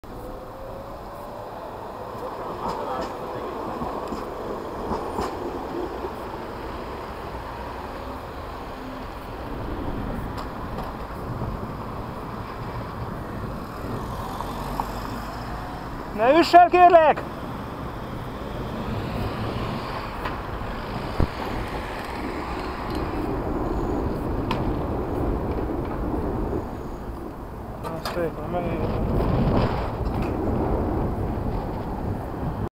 Ne üss el! PVD-236 + útonálló
Amelyik irányból nem jöhet autó, arra minek is nézni. El sem mozdította a fejét balra. Vajon észrevette volna a villamost, ha visszafogva közlekednek? Az útonálló meg ismét rákezdte.